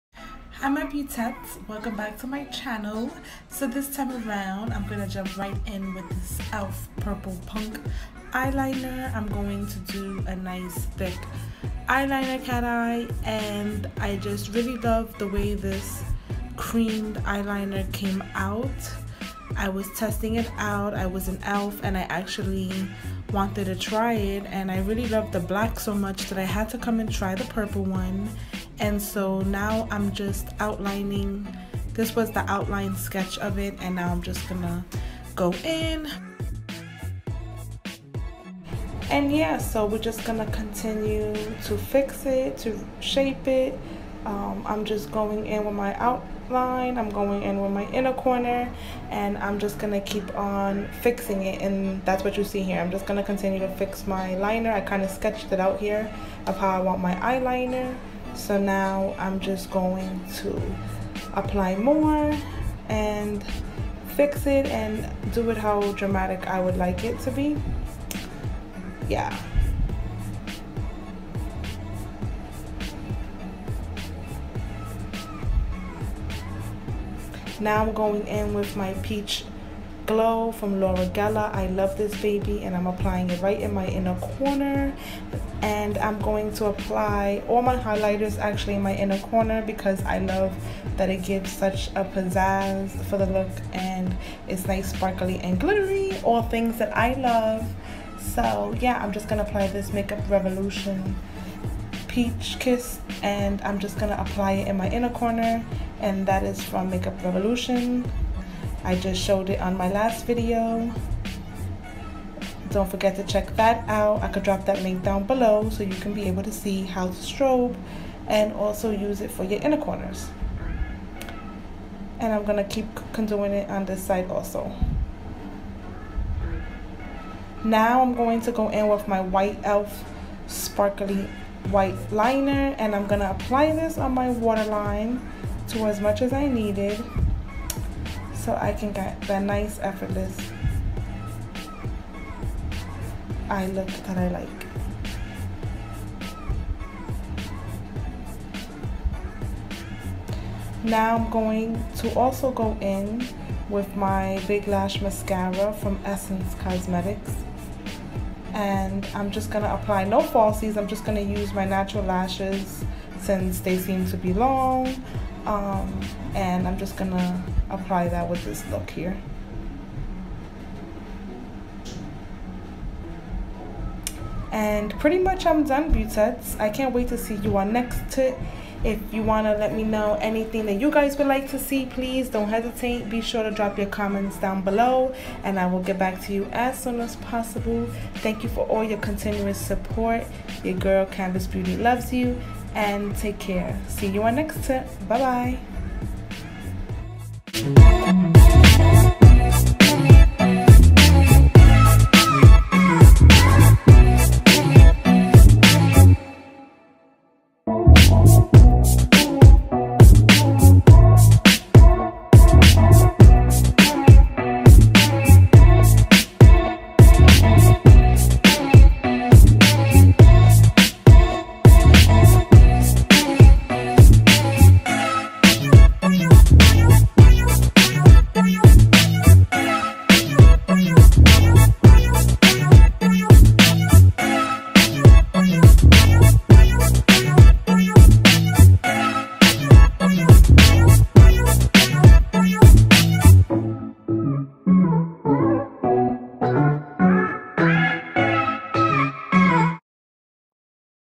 ELF Purple Makeup Tutorial | Canvas Beauty
Hii my beautets 👋🏻 Hope you like this quick purple hook eyeliner . If you like to see more drop your comments down below . Can't wait to see you beauties on my next Tutorial .

Products used :

Elf gel purple eyeliner "Purple Punk "

Elf sparkly white eyeliner

Laura geller illuminator "Peach glow "

Elf Purple LipGloss